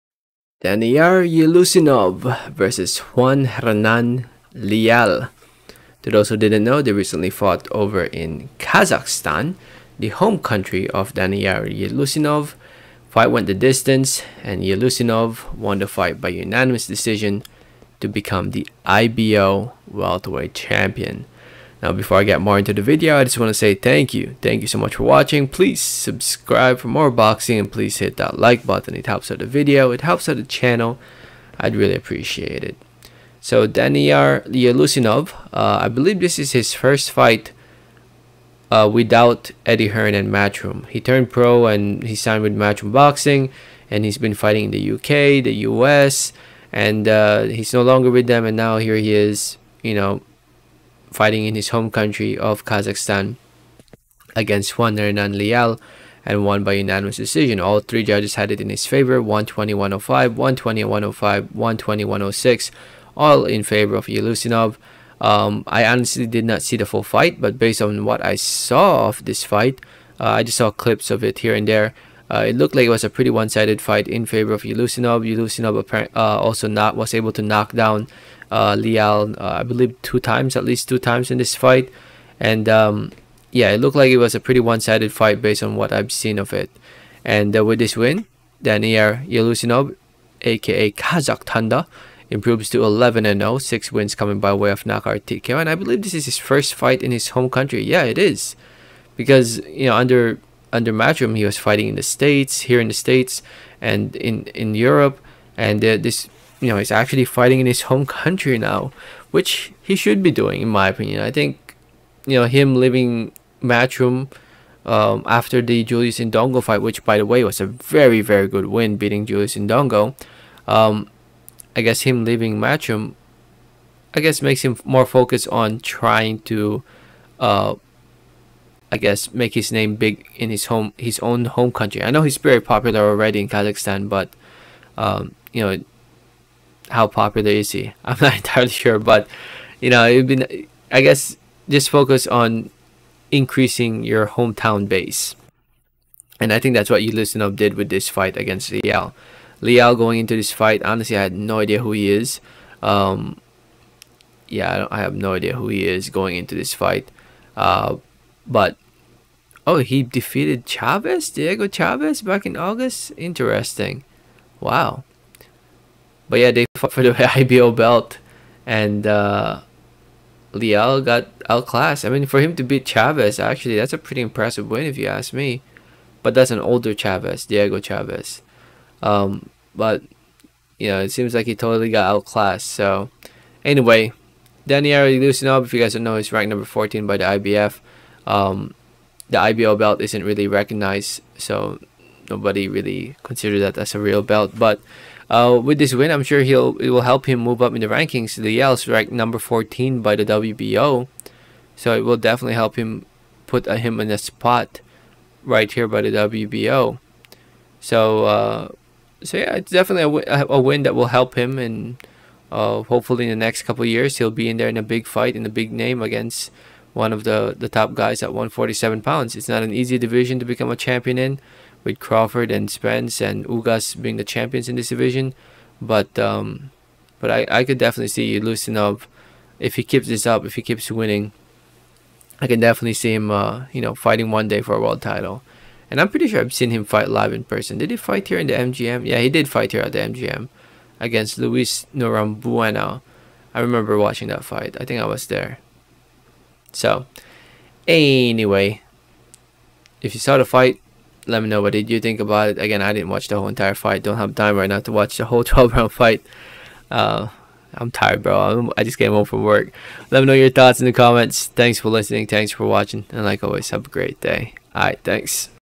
DANIYAR YELEUSSINOV VS JUAN HERNAN LEAL (UD 120-105x2, 120-106) YELEUSSINOV IS NOW THE IBO CHAMPION!
🥊 Boxing Fan
🎮 Gamer
🇵🇭 Filipino
🇺🇸 American
🌎 Earthling